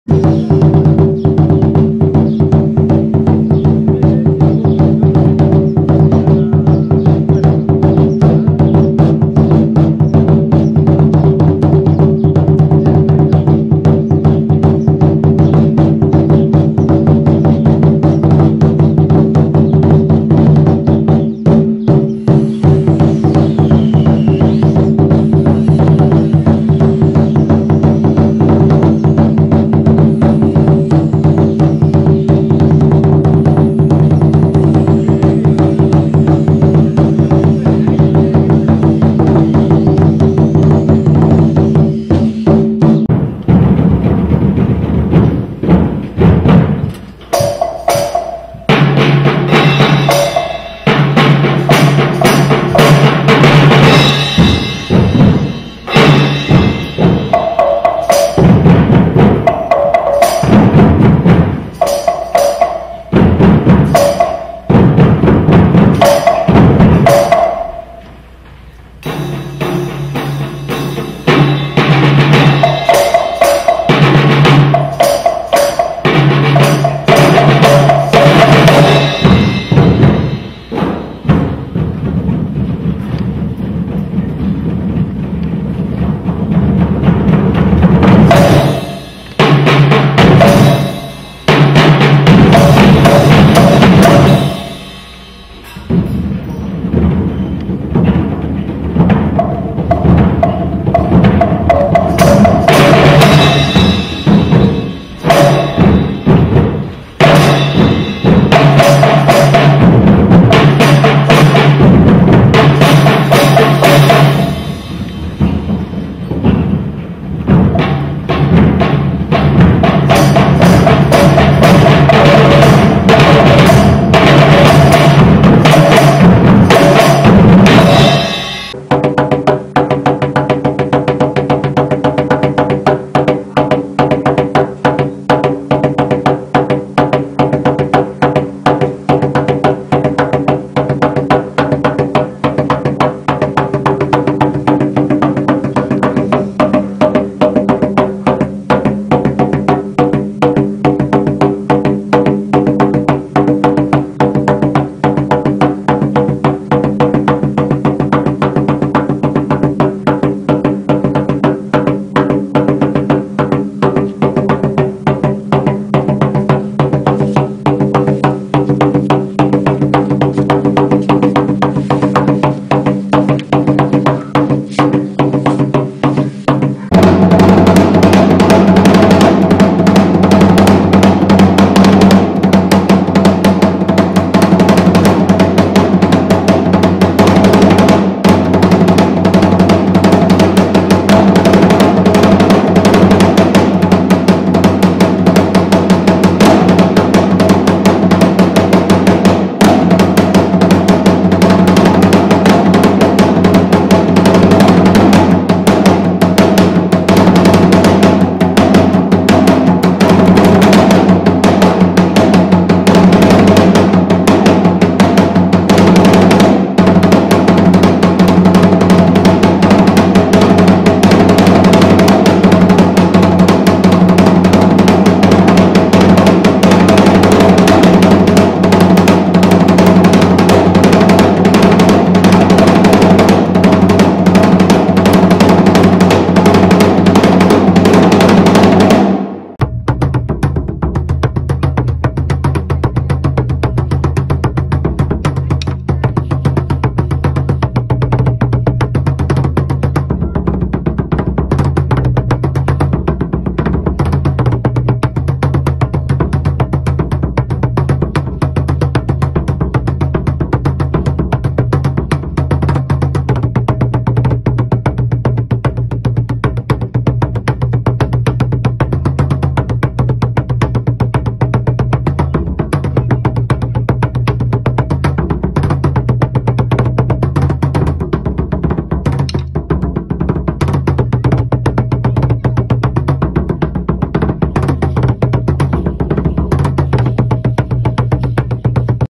Huehuetl Instrument#
is a percussion instrument from Mexico, used by the Aztecs and other cultures. It is an upright tubular drum made from a wooden body opened at the bottom that stands on three legs cut from its base, with skin stretched over the top. It can be beaten by hand or wood mallet.